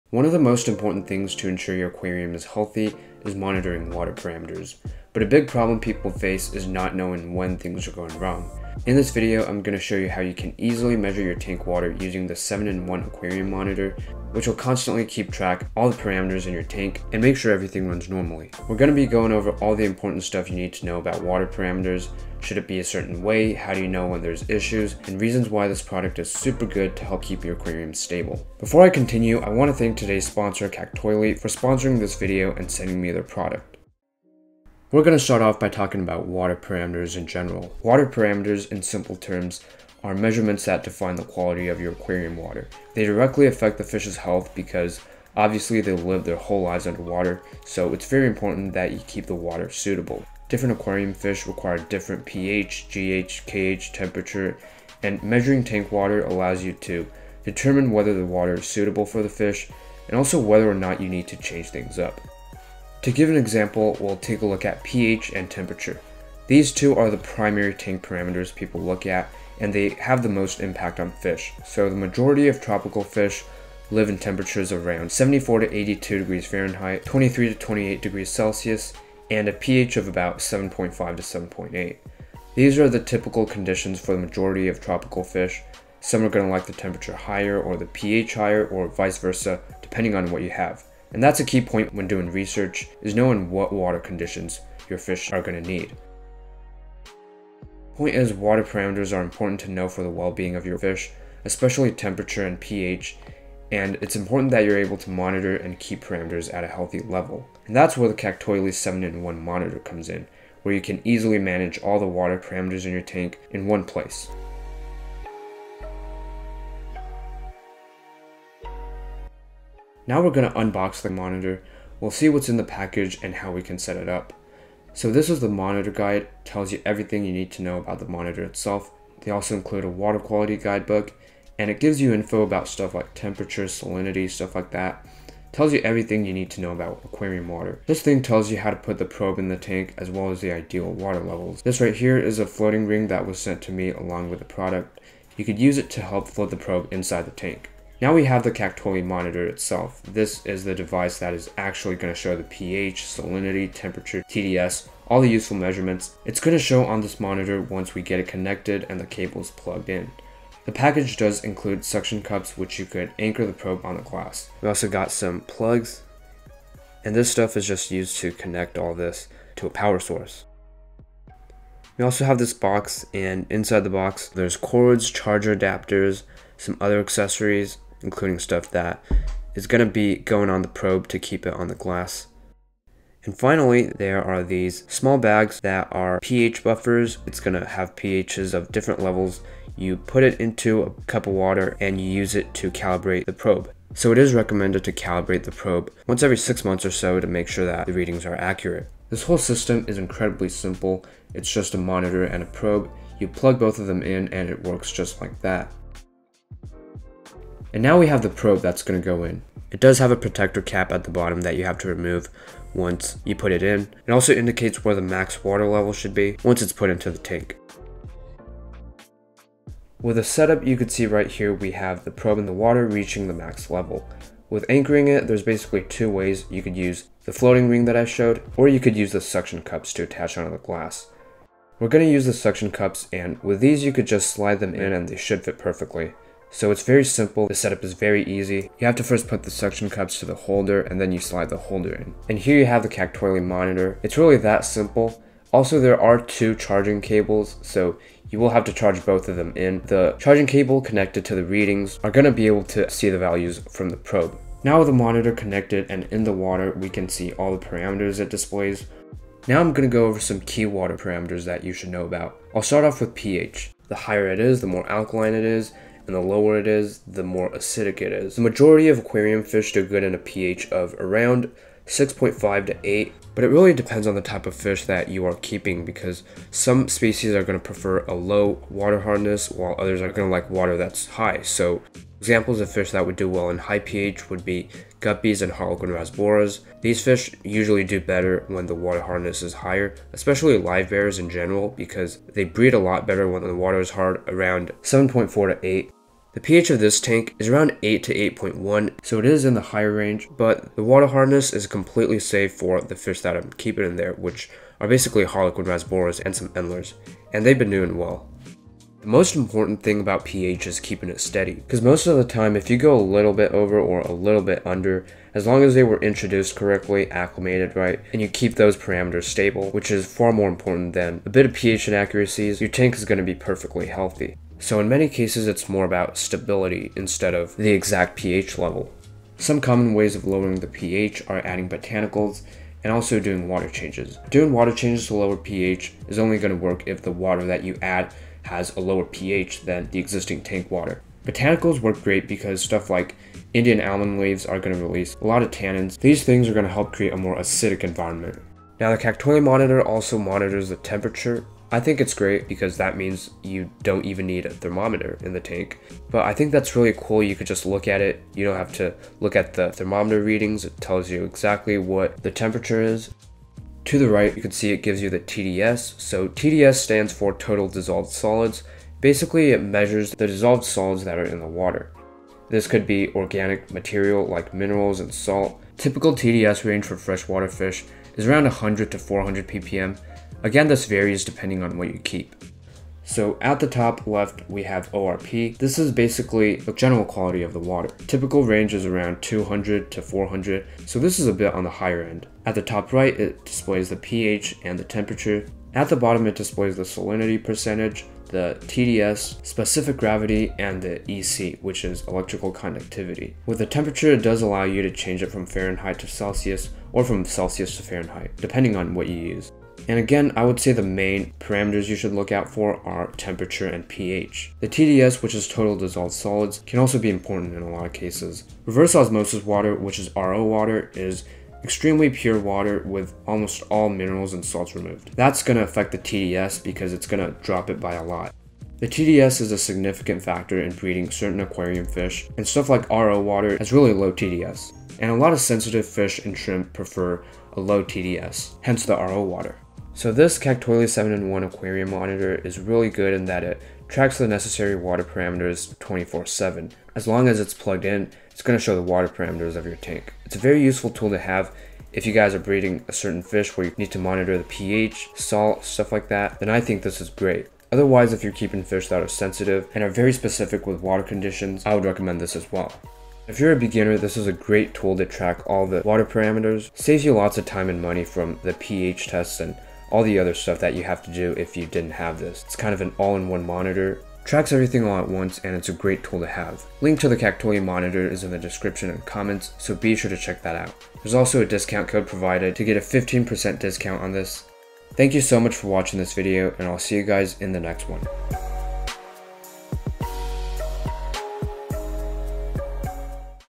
This Device Maintains Your Aquarium Water Parameters 24/7 | Kactoily 7-in-1 REVIEW
In this video, we take a look at the Kactoily 7 in 1 Aquarium Monitor which keeps track of key aquarium water parameters 24/7. This device displays live parameters of your aquarium water to ensure everything is at a normal level. It monitors pH, temperature, TDS, water hardness, ppm, and more. Works for freshwater as well as reef tanks.

00:00 Issues with water parameter maintenance
00:34 The reasons why water parameters are important
01:54 Kactoily 7 in 1 Unboxing and Key Features
03:34 Easy Monitor Setup
04:20 Important Water Parameters it Measures
07:12 Kactoily 7 in 1 Overview Summary